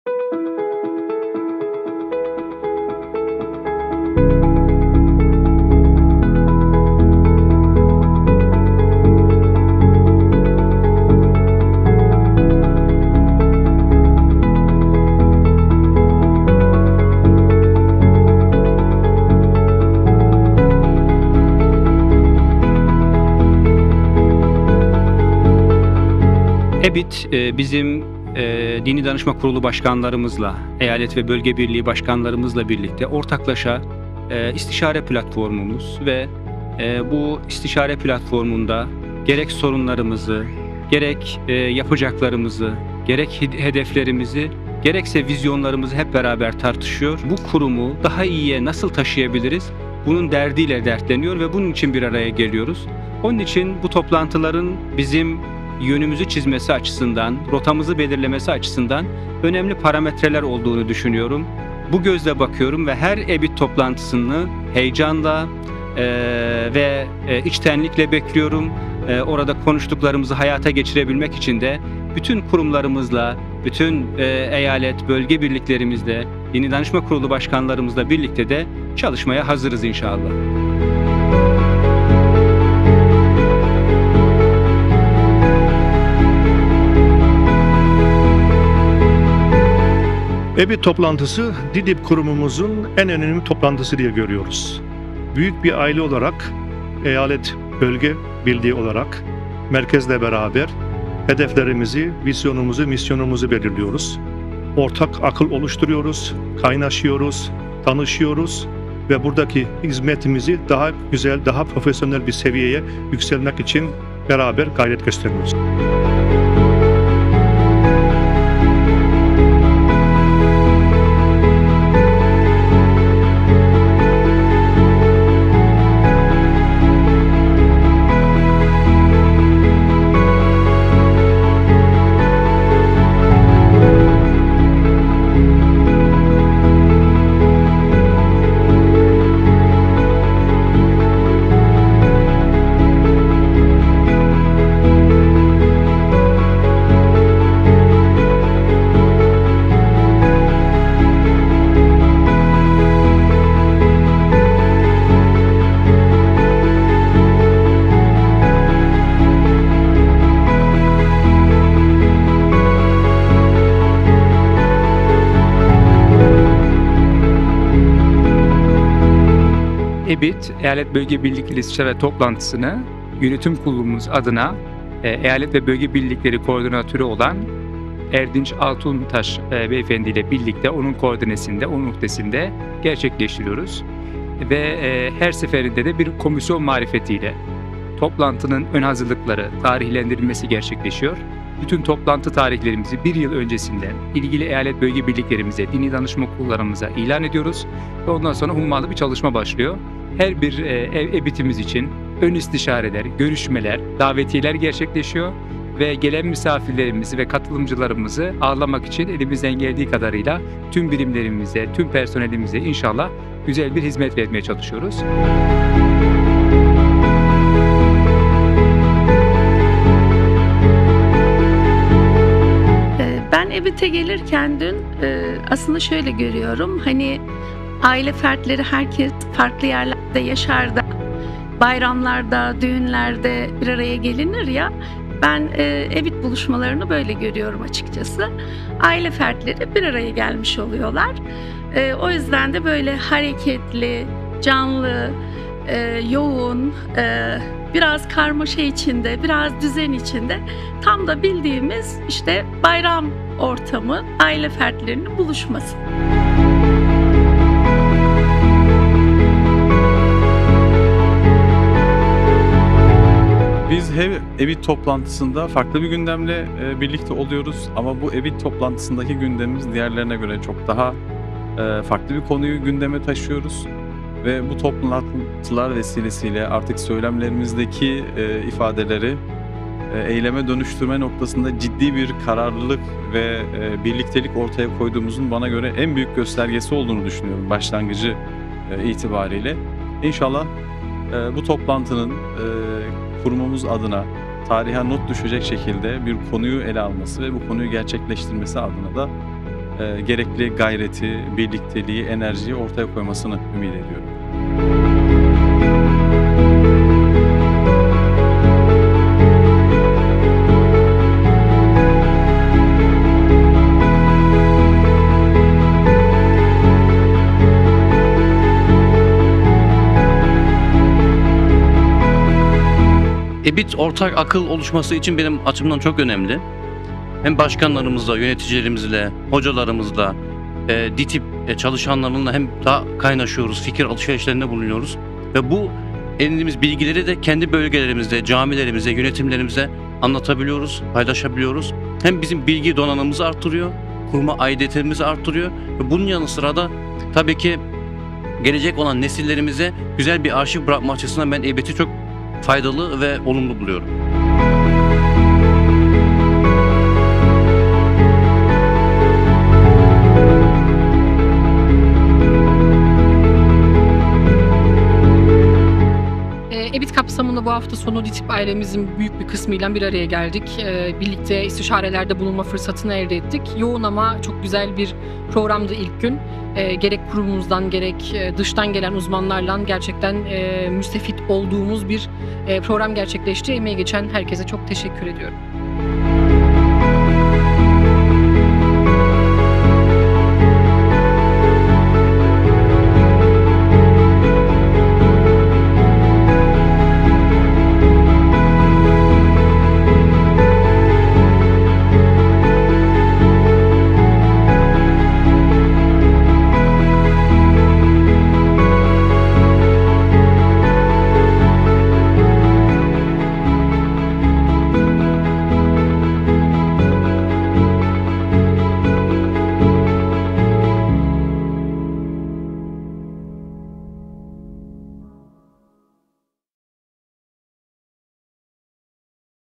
EBİT 2023
Diyanet İşleri Türk İslam Birliği (DİTİB) Federal Birliği, 16/17 Eylül 2023 hafta sonu Köln Merkez Camii’nde düzenlenen yıllık istişare toplantısı vesilesiyle tüm DİTİB eyalet birlikleri ile bir araya geldi.

Toplantıda DİTİB Eyalet Birlikleri, DİTİB’in dini bir cemaat olarak devlet okullarında İslam din dersi veya üniversitelerde İslam ilahiyatı gibi devlet kurumları ile ortak olduğu eyaletlerdeki mevcut durumu ve hizmetleri istişare etti. Bunun yanında DİTİB’in eyaletlerdeki danışma kurulları veya komisyon modelleri gibi geçici çözümlere katkıda bulunduğu gelişmeler de istişarelerin konusu oldu.